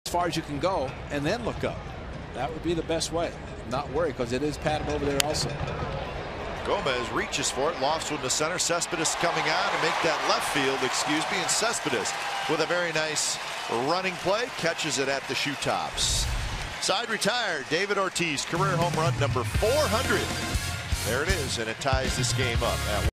2012/07/04 Cespedes' great catch
BOS@OAK: Cespedes makes a shoestring grab in left